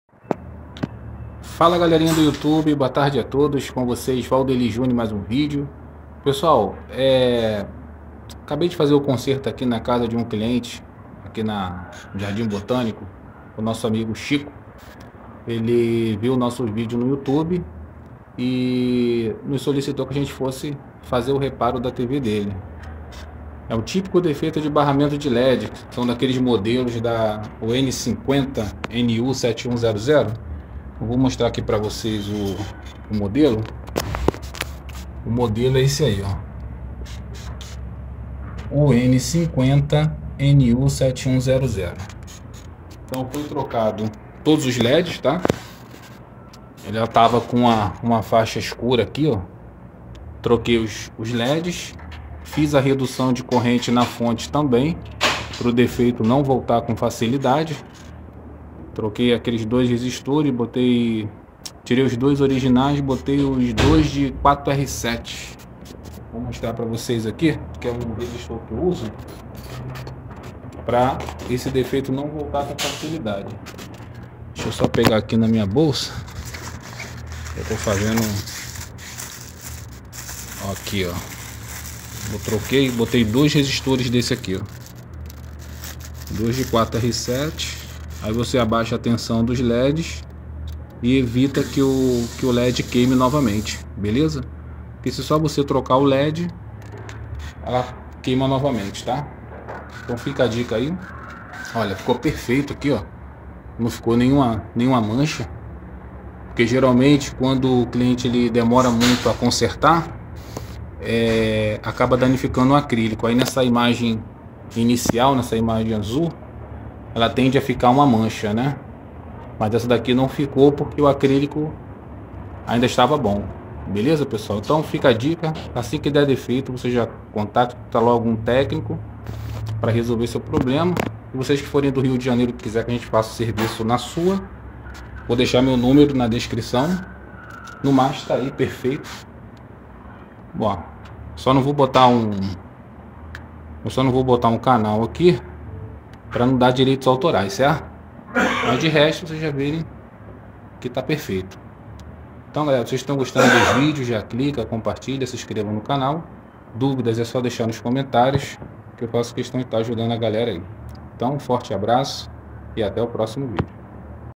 tv Samsung com faixa preta na tela
Resolvido com a troca dos leds, fiz o serviço na residência do cliente no jardim botânico, meu contato pra quem quiser o serviço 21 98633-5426 ZAP